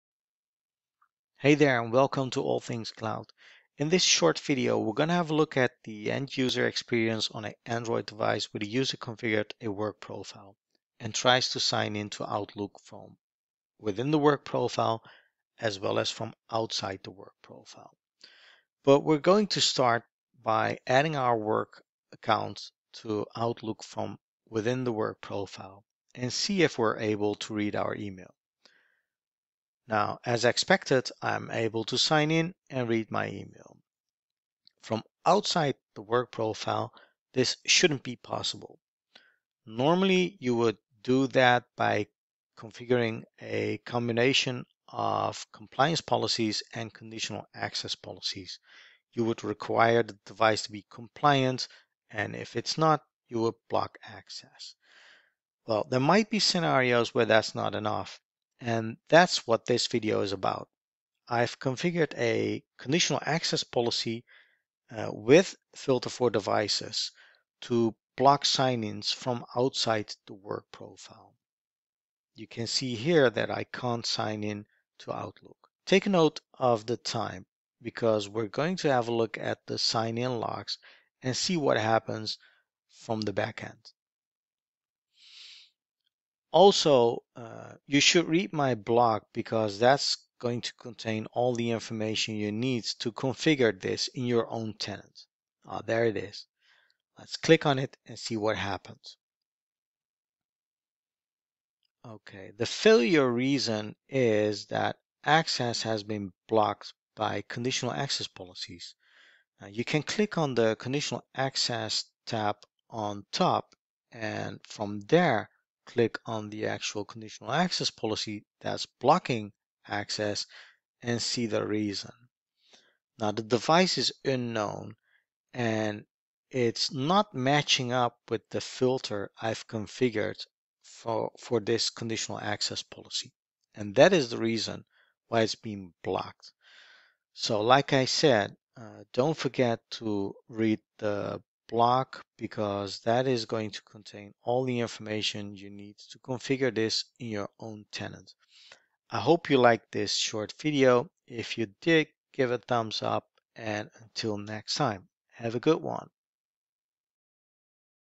Blocking access to Microsoft 365 outside the Android for Work Profile with Endpoint Manager
This short video shows the end-user experience on a Android device where the user configured a work profile. It shows that access to Microsoft 365 outside the Android for Work Profile is blocked. because there you'll have all the information to configure this in your own tenant.